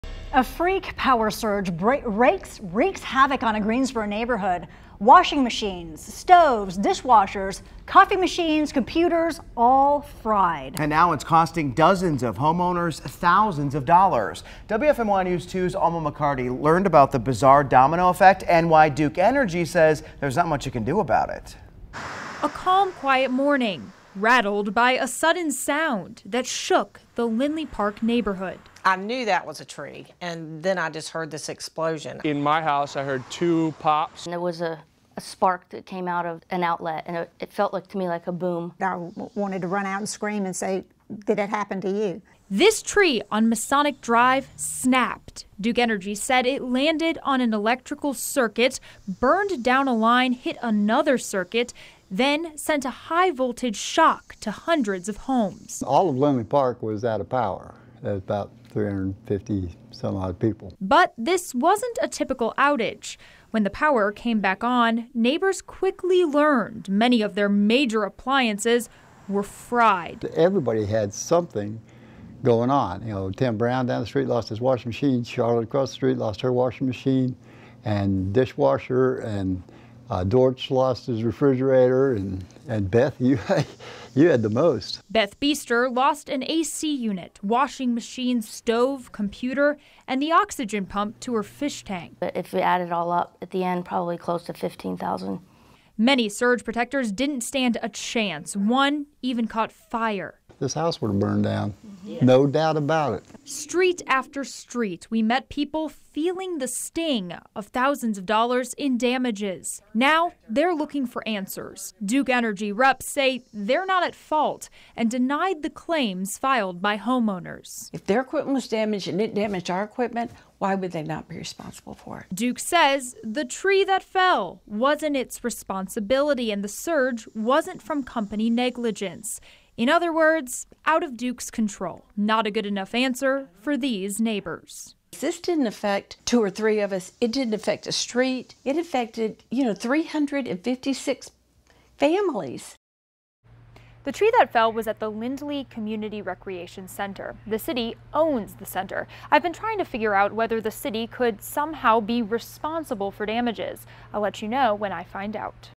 Neighbors In Lindley Park Must Pay Thousands After Power Surge Fries Their Appliances
A power surge rendered dozens of appliances useless in Greensboro neighborhood, and Duke Energy says they're aren't responsible for paying for it.